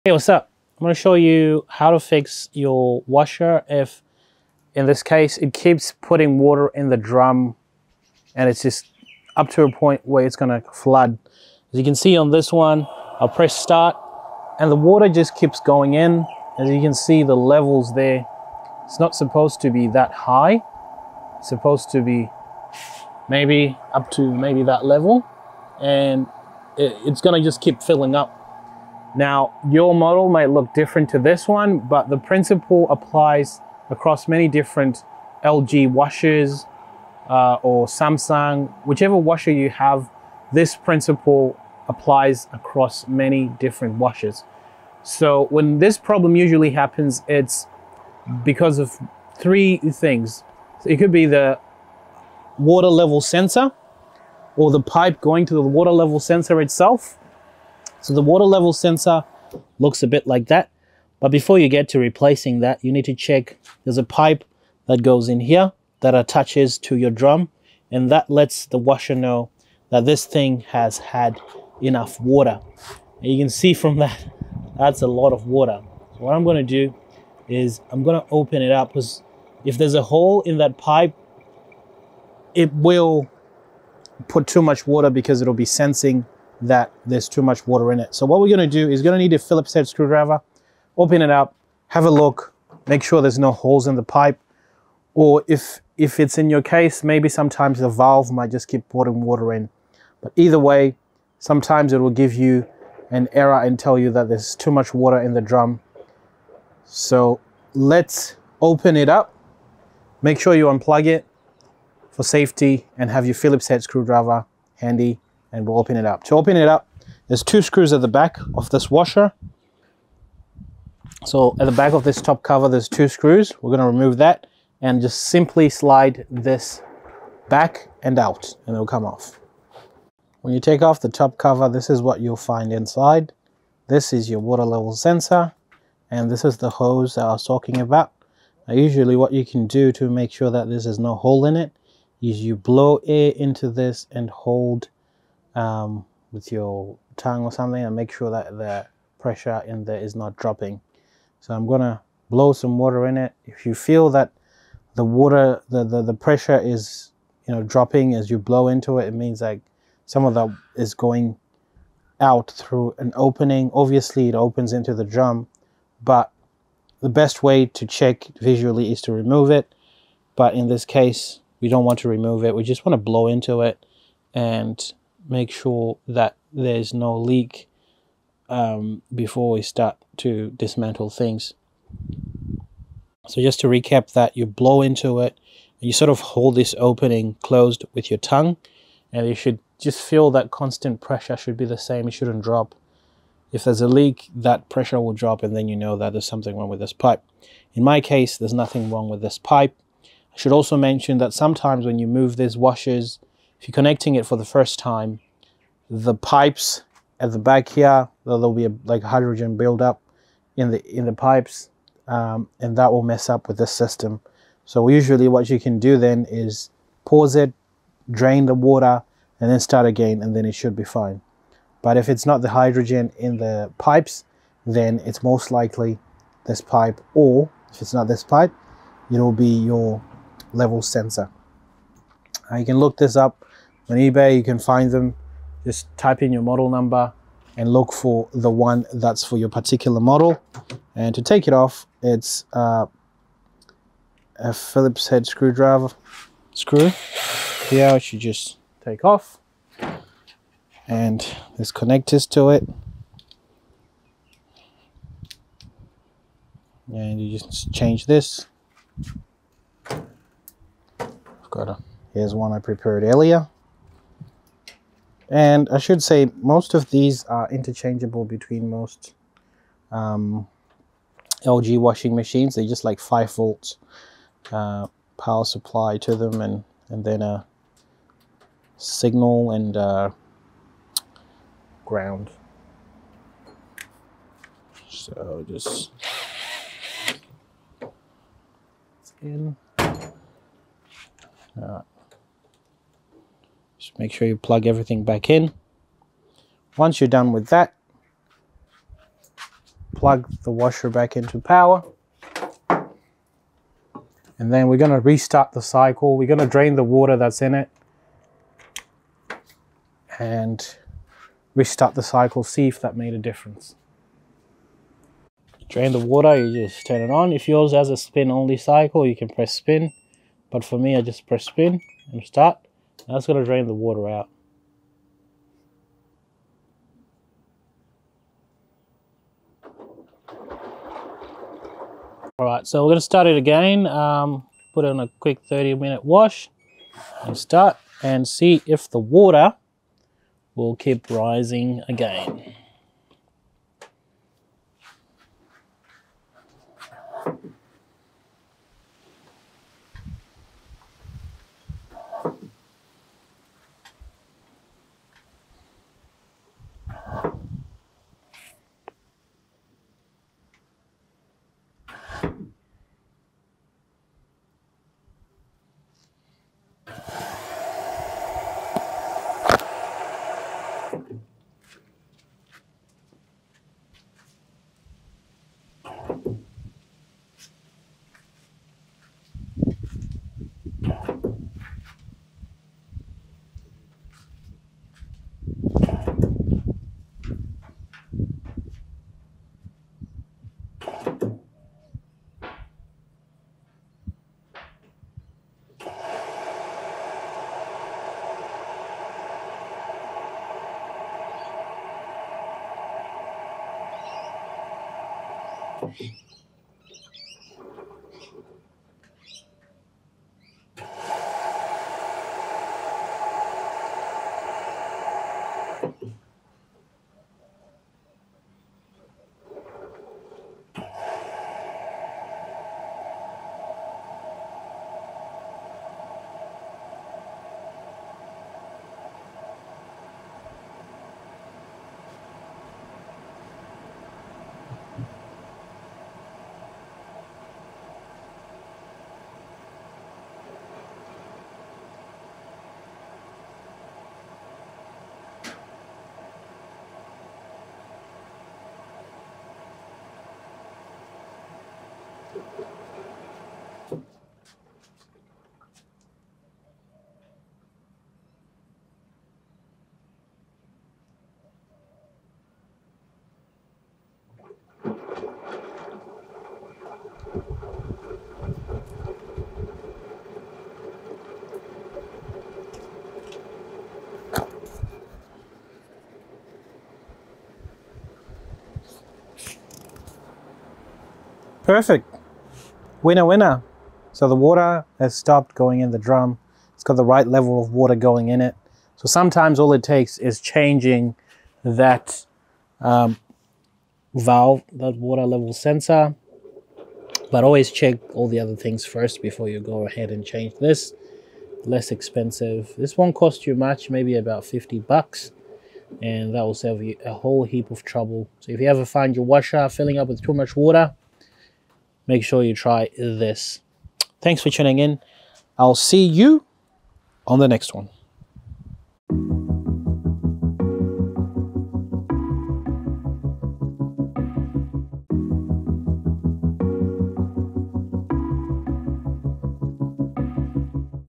Washing Machine Keeps Filling Water Over Fills Repair
A quick tutorial on how to fix a washing machine that overfills with water. Applies to most front  load washing machines.